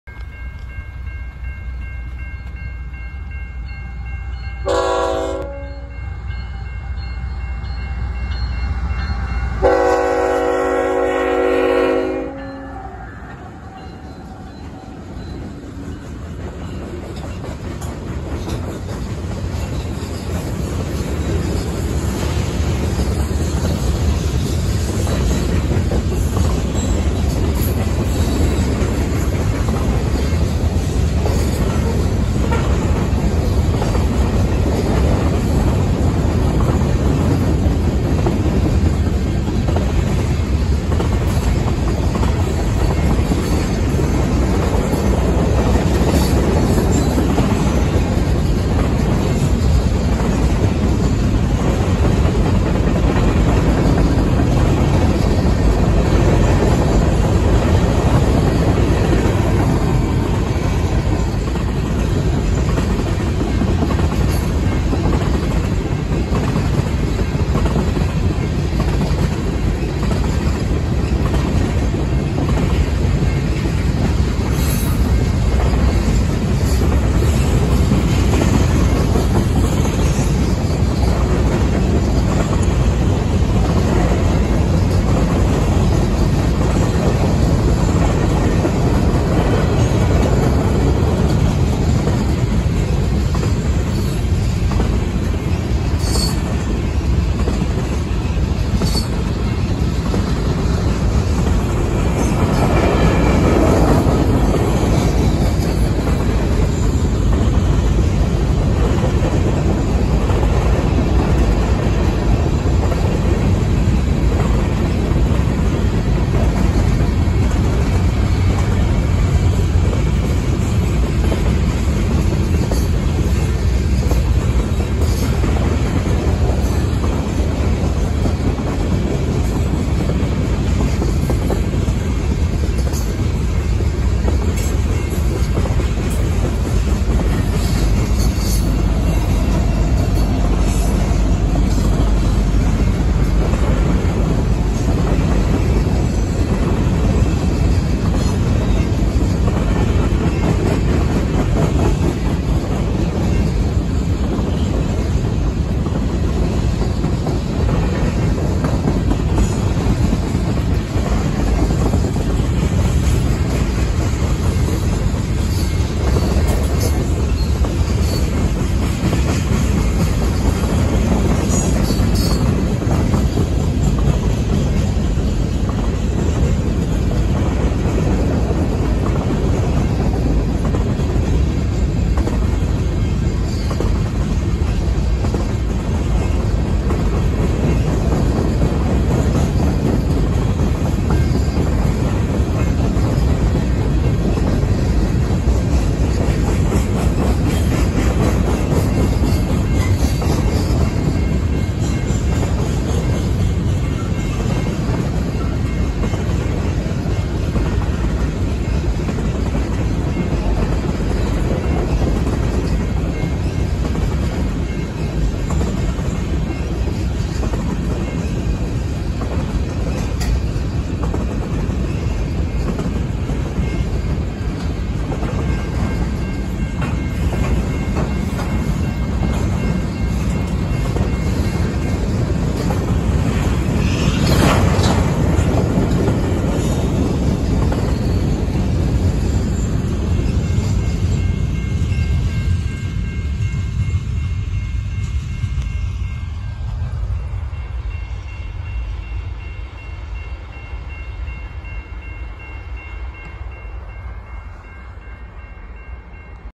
CN Mixed Freight Pushes North
Train heading northbound just north of the Quad plant near Lomira, WI. Taken looking east on County Rd Y at 1925 CST 31-JUL-2021